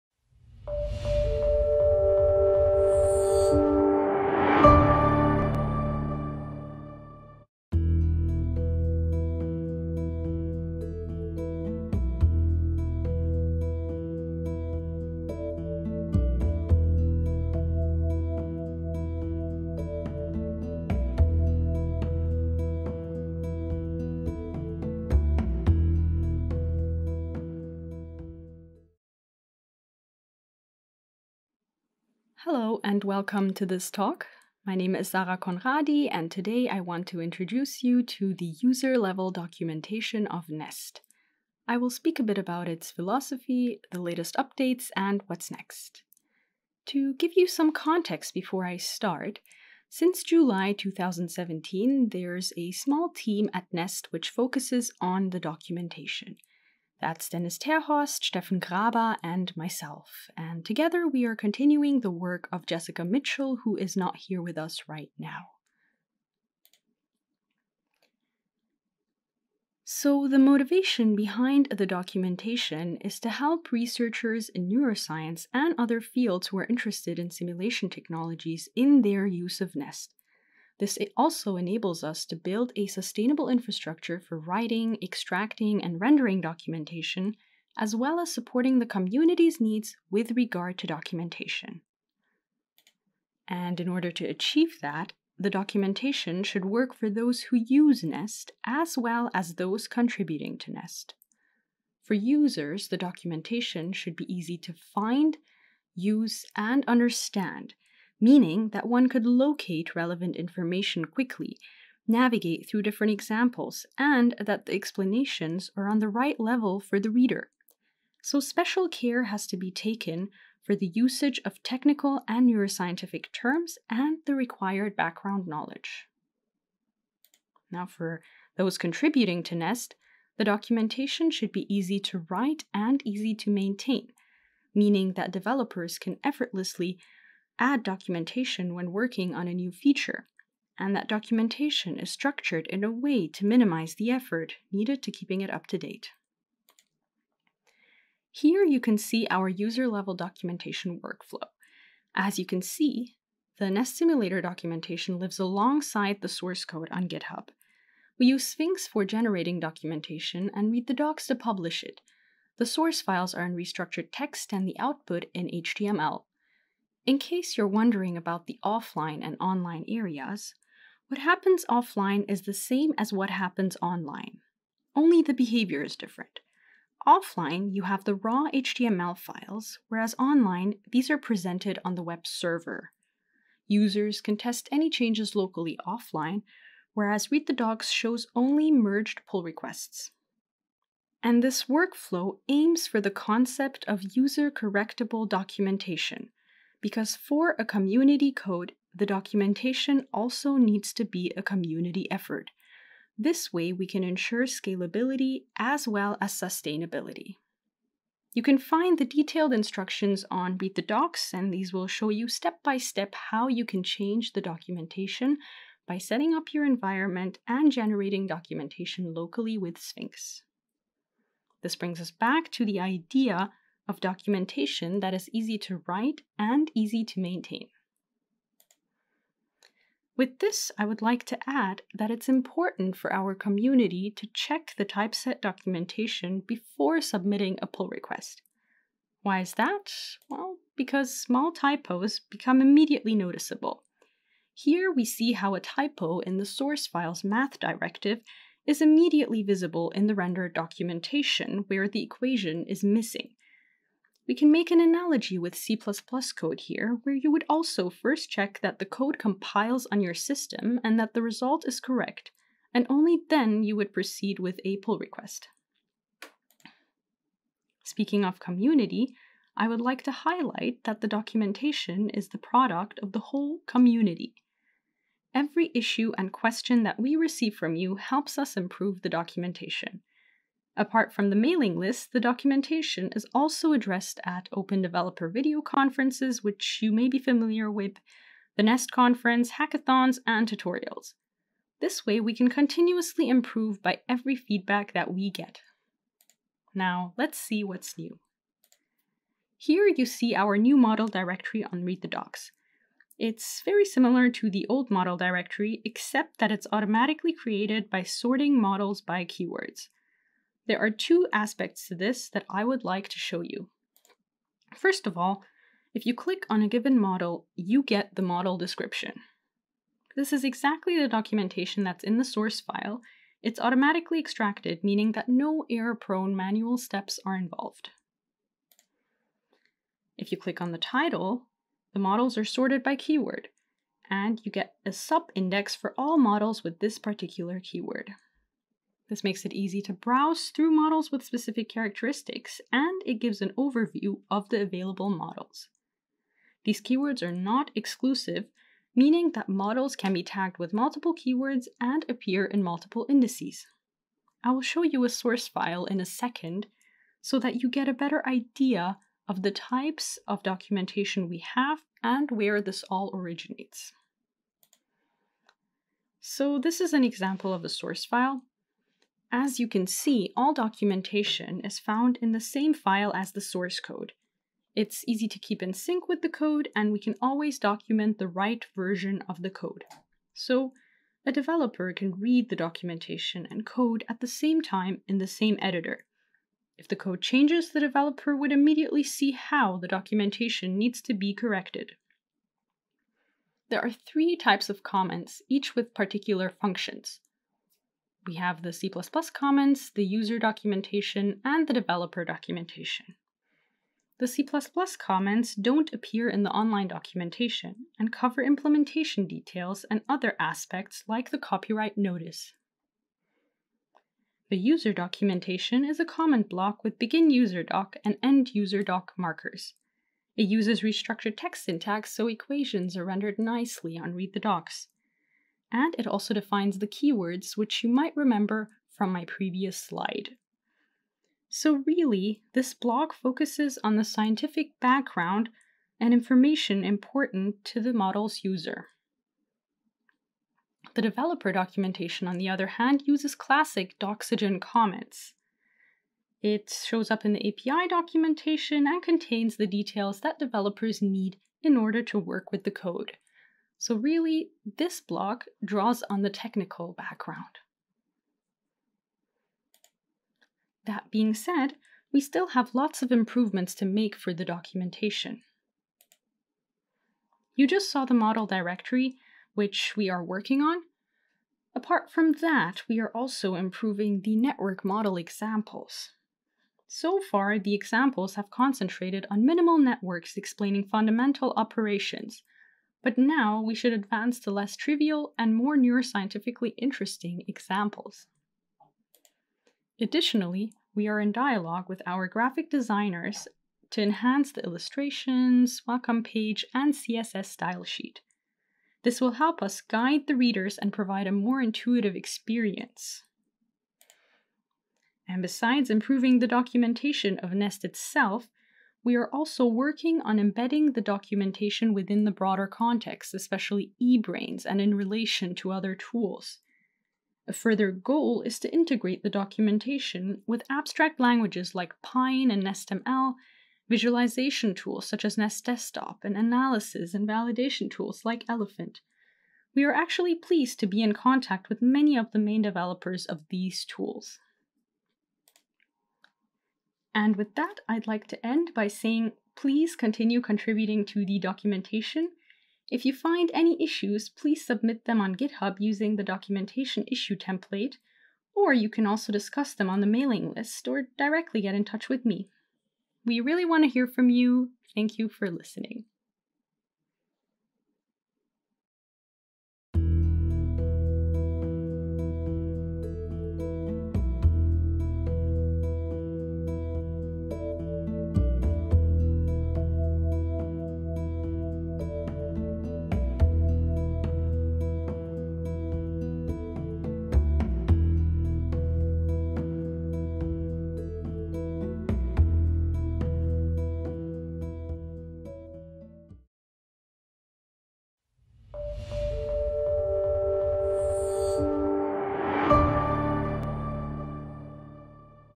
NEST user-level documentation: Why, how and what's next?
Computational neuroscience strongly depends on the usage of a multitude of software tools, and the correct use of these directly impacts scientific results. Good documentation is therefore vital for understanding the tools' functions and capabilities. This video introduces a concept for user-level documentation that is based on the neuronal network simulator NEST.